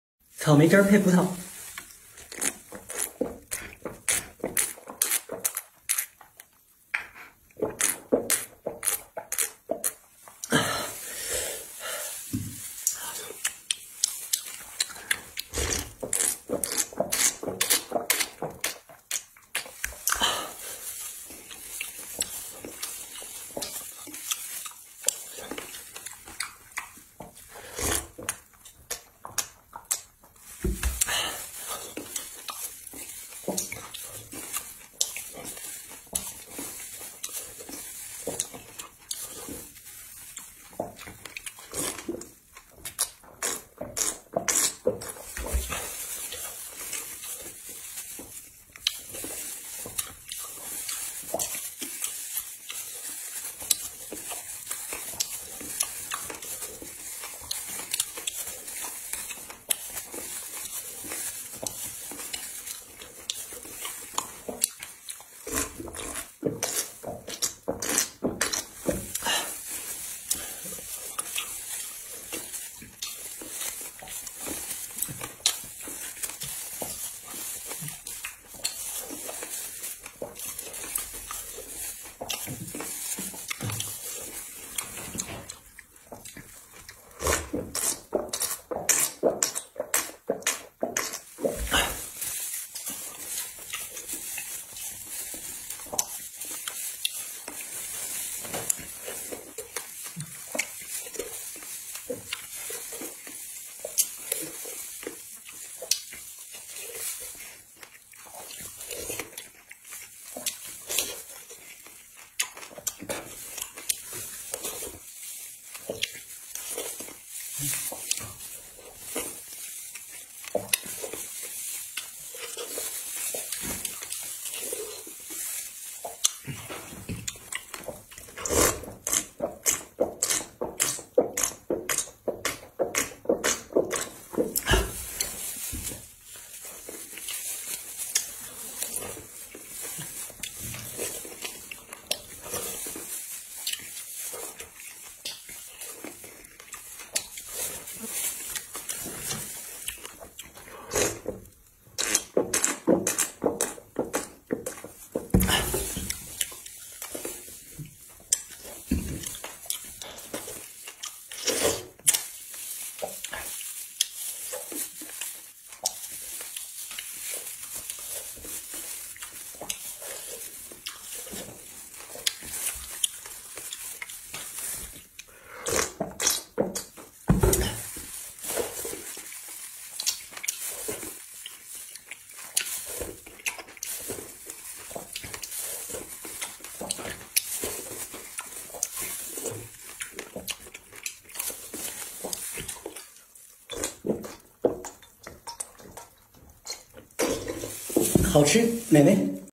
MUKBANG; WATCH YOU DRINK THIS HUGE pan of water and grapes TODAY🍇
mukbang: see if you drink this huge pan of grape juice, water is very necessary for the body, grapes have many nutrients and vitamins to provide abundant health, so you ate it all ok Older brother